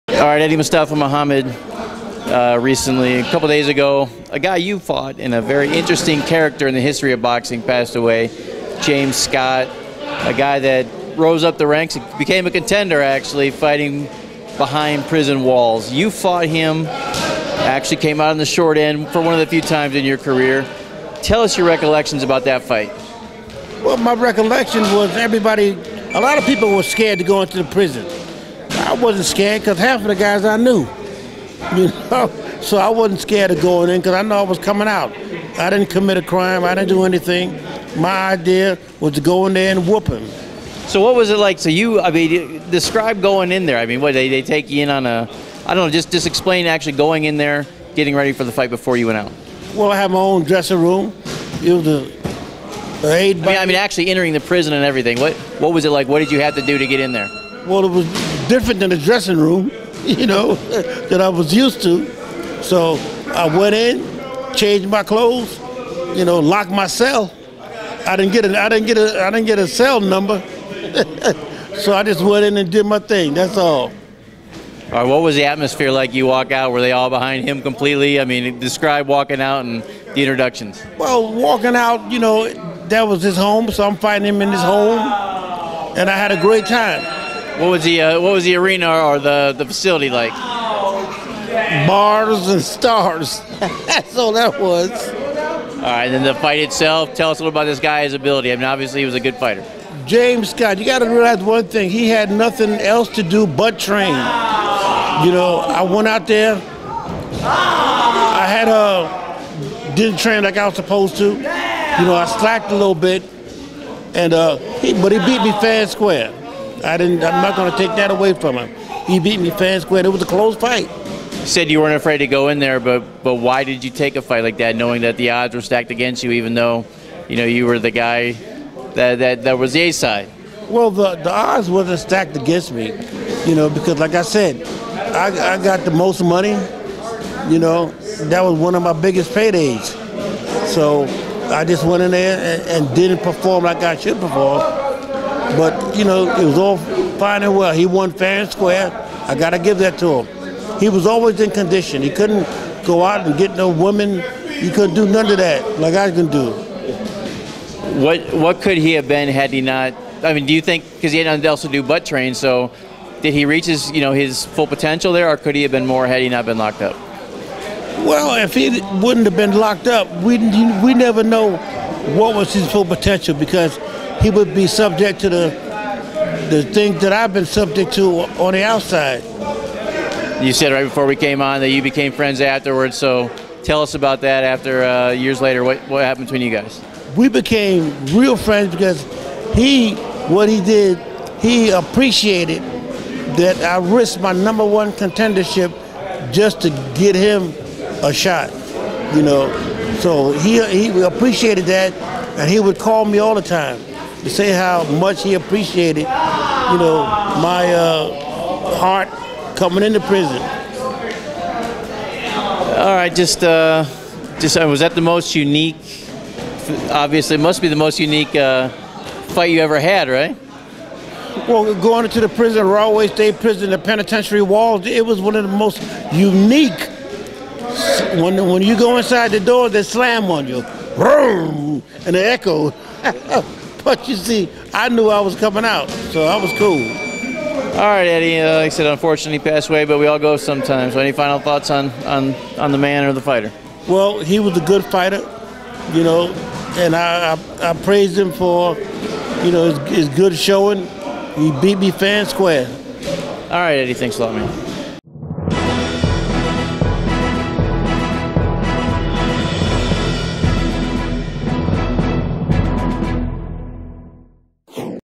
Eddie Mustafa Muhammad on the passing of James Scott, as well as their prison fight
Eddie Mustafa Muhammad talks about the passing of James Scott as well as talks about their fight behind bars